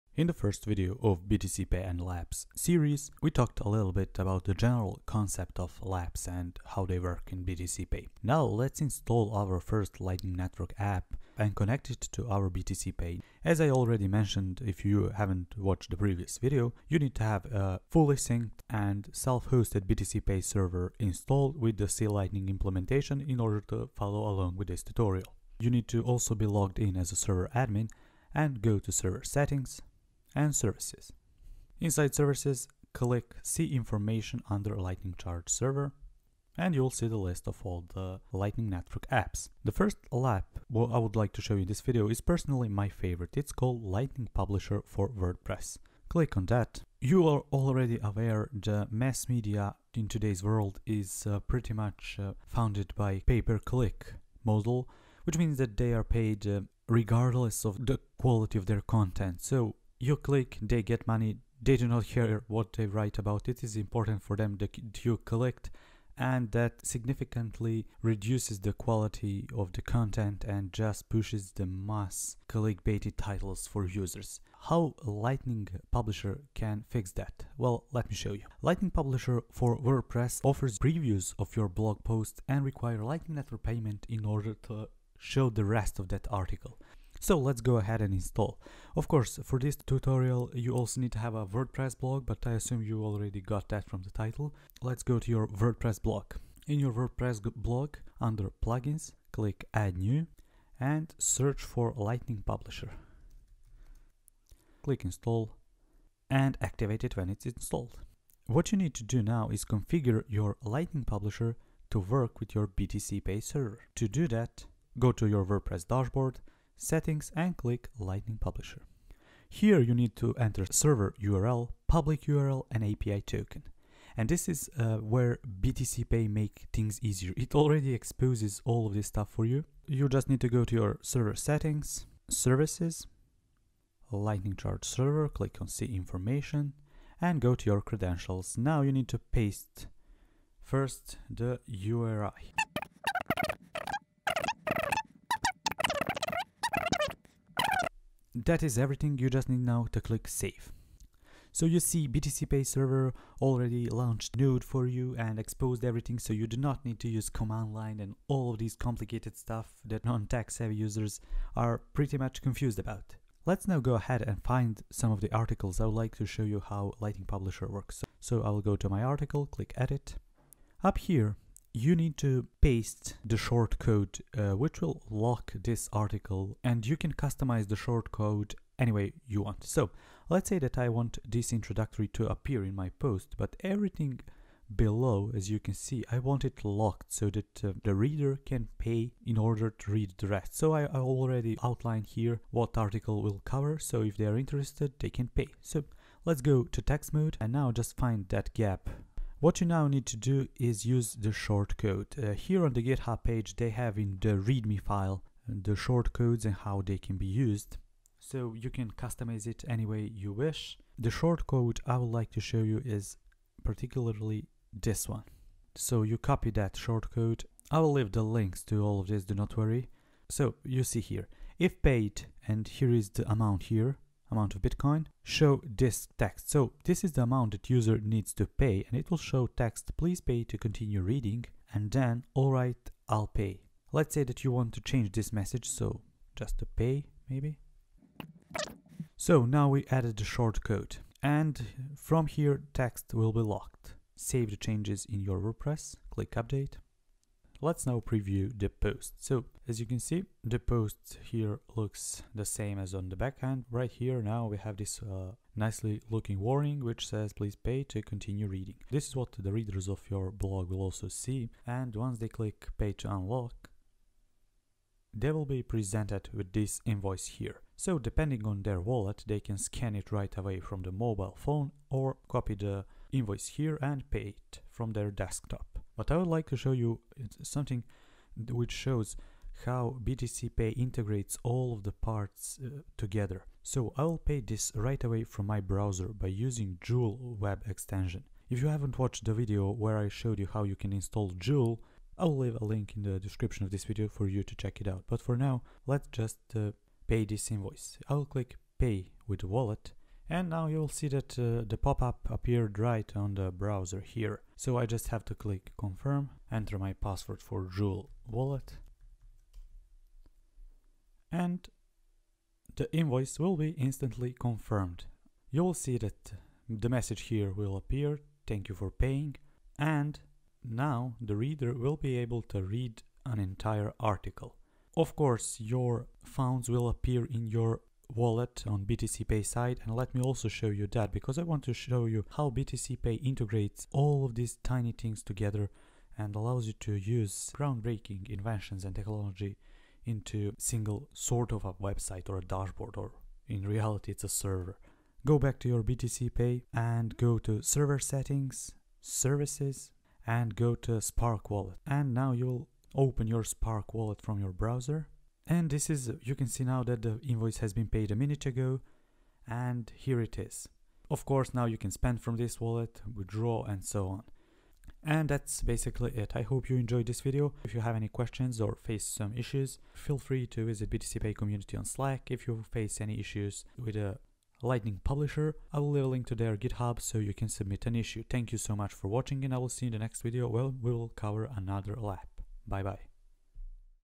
Lightning Wordpress Publisher and BTCPay ( Bitcoin Micropayments for Blogs, News sites)
This integration between BTCPay Server and Lightning Publisher for WordPress allows you to hide your articles behind a paywall where the reader is required to pay in order to unlock the article.

The ability to do micropayments with Lightning Network is one of the most important innovations that can potentially improve the content we consume online.

BTCPay bundles everything into one coherent and easy to use application an exposes your Lightning Network node info that the WordPress plugin requires.

► If you don't have a BTCPay server, you can use the 1-click web-deployment or if you have technical knowledge take a look at our extensive documentation